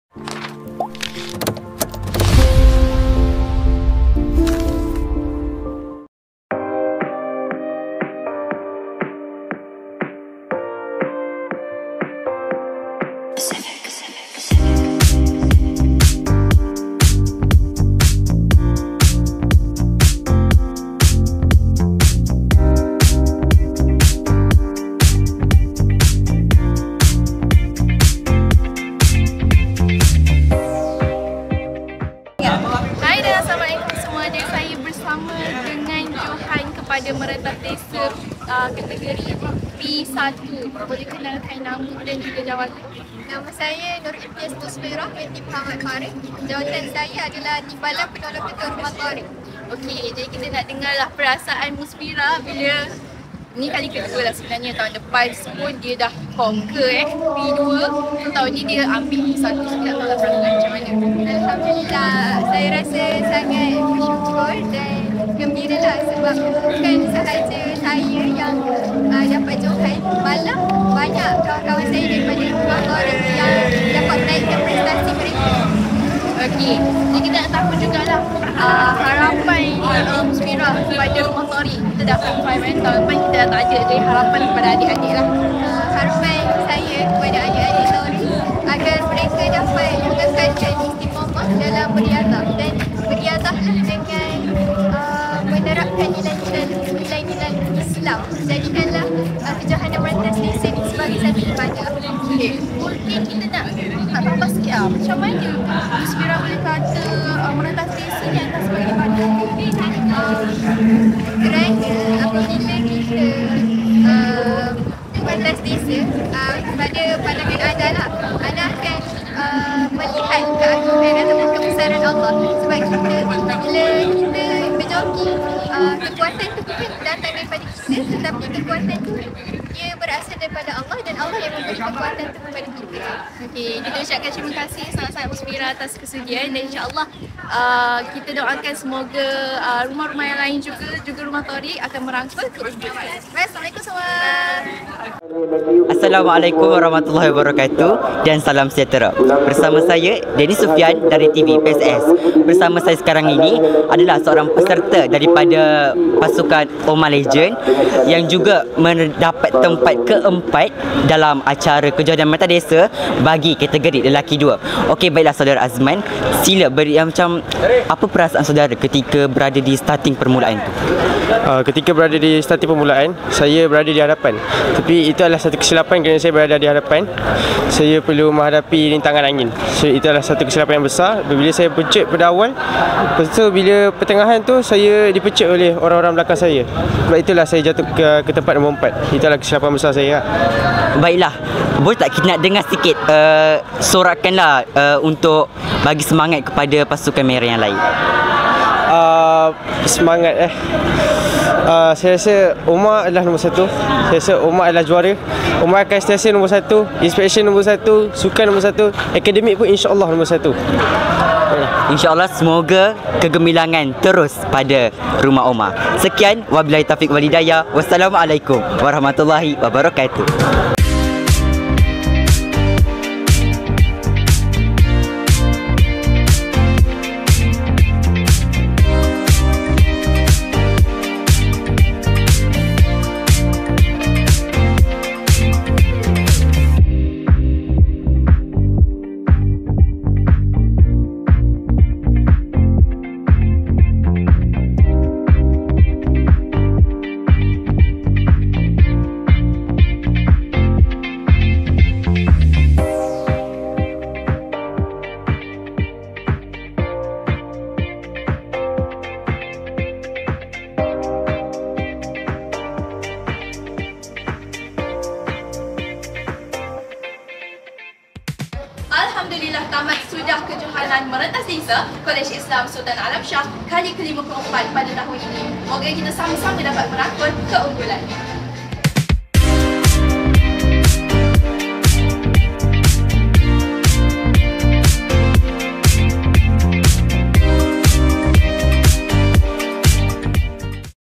Episod 49: Kejohanan Merentas Desa (2023) | Siri Lensa KISAS
Jom saksikan rangkuman Kejohanan Merentas Desa yang telah dijalankan di sekolah pada sesi 2023 !

Dibawakan Khas oleh TVPSS KISAS

Kredit:
Penyunting video: Qistina Nur Tasnim

Jangan lupa !